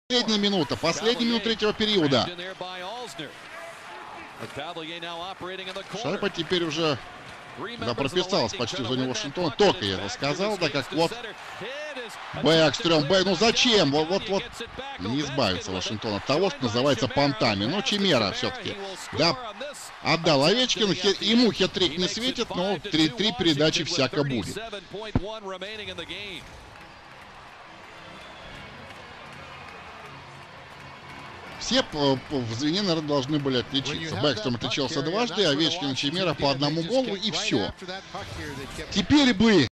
[HD] Goal Jason Chimera (Washington & Tampa Bay) NHL 02/5/11
5-2 Goal Jason Chimera (8) (Empty Net)
Assists: Alex Ovechkin-35 , Nicklas Backstrom-36